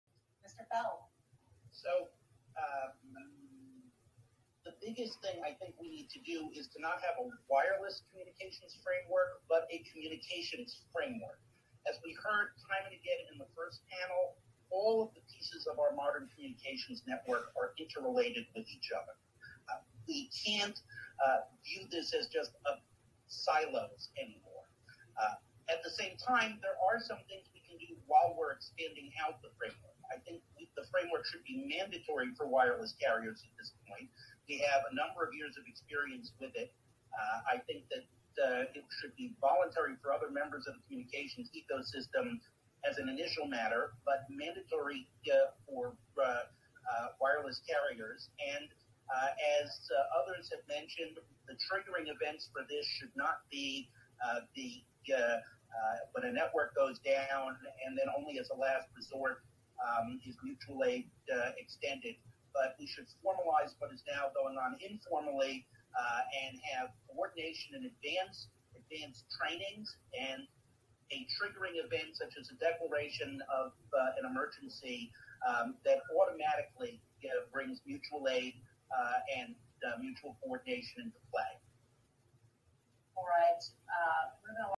FCC Disaster Communications Field Hearing: Chairwoman Rosenworcel questioning SVP Harold Feld
Senior Vice President Harold Feld responding to a question by Chairwoman Jessica Rosenworcel about our interconnected modern communications network